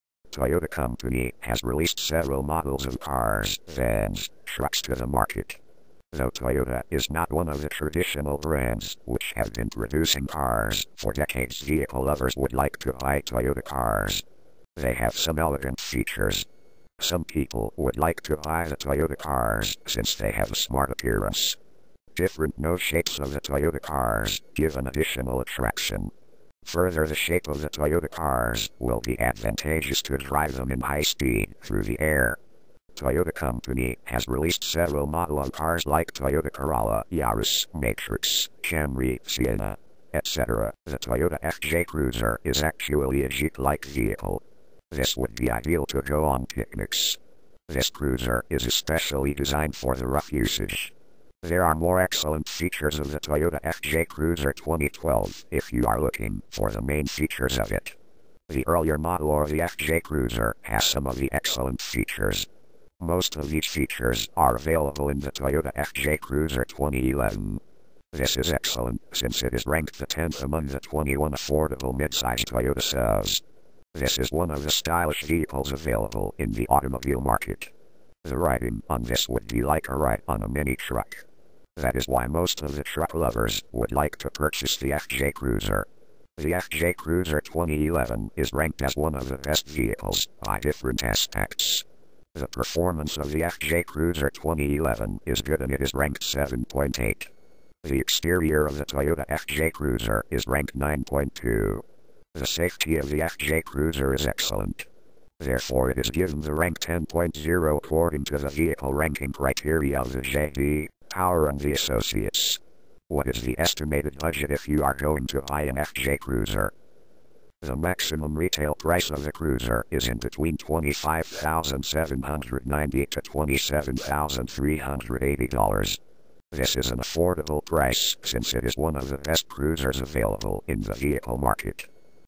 Toyota FJ Cruiser - A Photo Slide Show of the Latest Toyota FJ Cruiser 2012
The Toyota FJ Cruiser 2012 is a great redesign of an amazing vehicle.